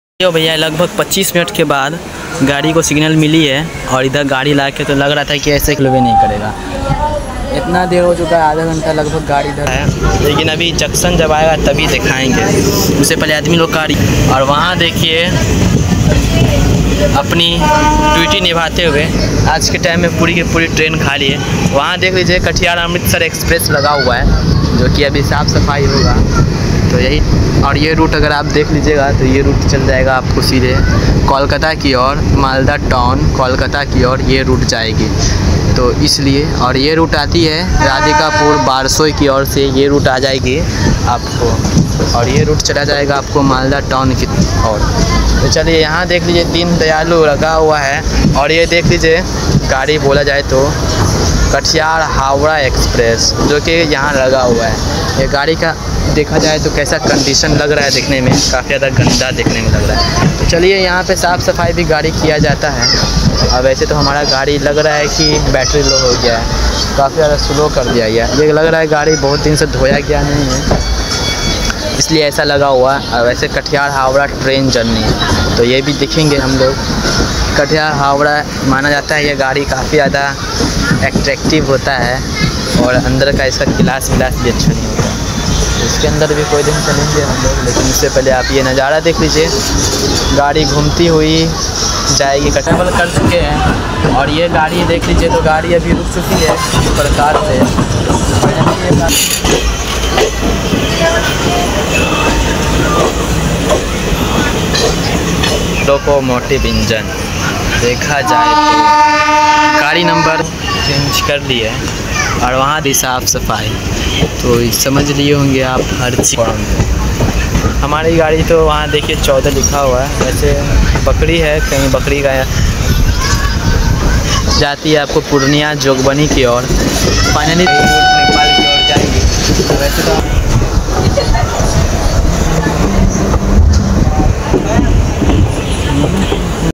Journey In General Coach Of Ajmer Sealdah Express 😱🤔!!
The 12987 / 12988 Ajmer–Sealdah Express is a Superfast Express train belonging to Indian Railways that runs between Ajmer Junction and Sealdah in India.

It operates as train number 12988 from Ajmer Junction to Sealdah and as train number 12987 in the reverse direction.